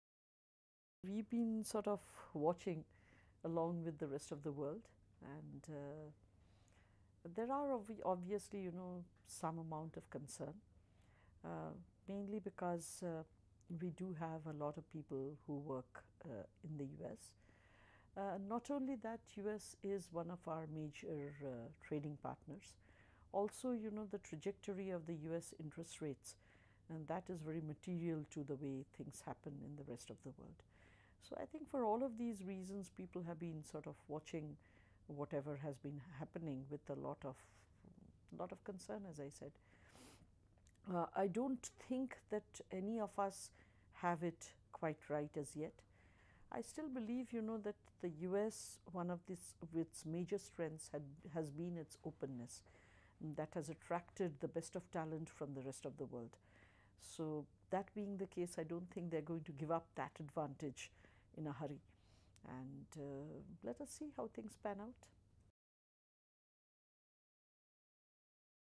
Indian banker watching Trump with 'concern'
Arundhati Bhattacharya, Chairperson of the State Bank of India, told CNN's Mallika Kapur that they are "watching, along with the rest of the world" the political and economic developments in the United States under new president Donald Trump.